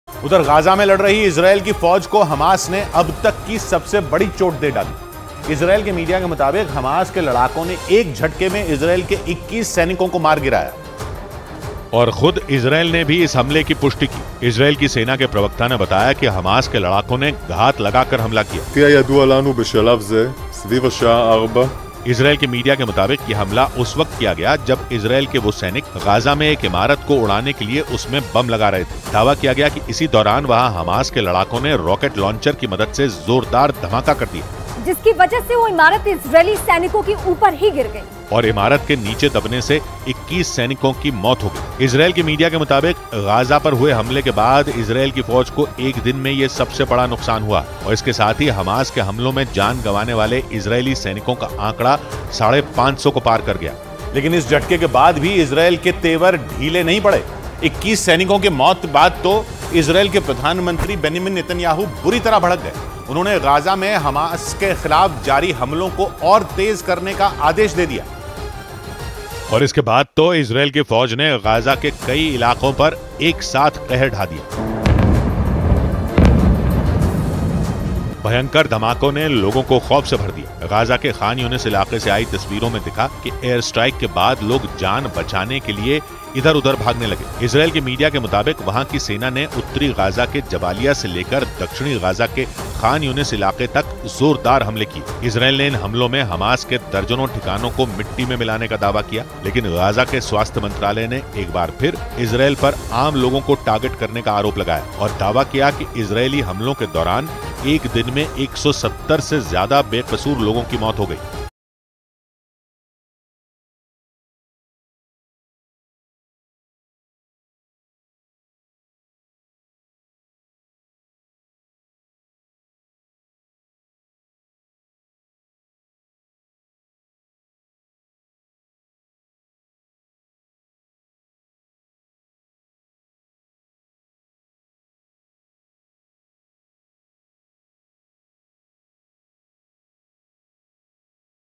Hamas ने Israel Army को Gaza में घेरकर मारा ? War | Palestine | Top News | Latest News | News18
ग़ाज़ा में लड़ रही इज़राइल की फ़ौज को हमास ने अब तक की सबसे बड़ी चोट दे डाली। इज़राइल के मीडिया के मुताबिक़ हमास के लड़ाकों ने एक झटके में इज़राइल के 21 सैनिकों को मार गिराया।